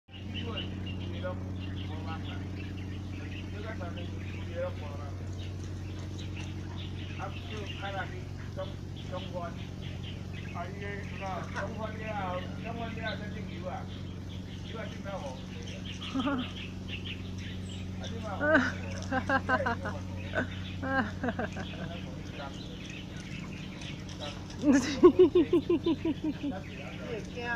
Sir Baker in a vicious standoff with the security guard's puppy
Tamsui, Taiwan